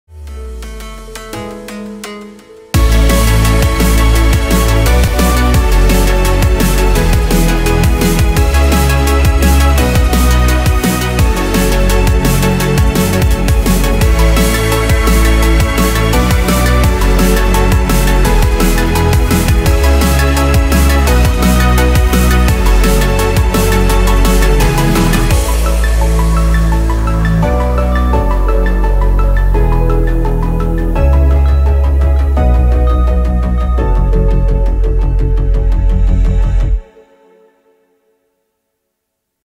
Genesys Cloud CX: Educação #1
Impacto devido à falta de digitalização para instituições de ensino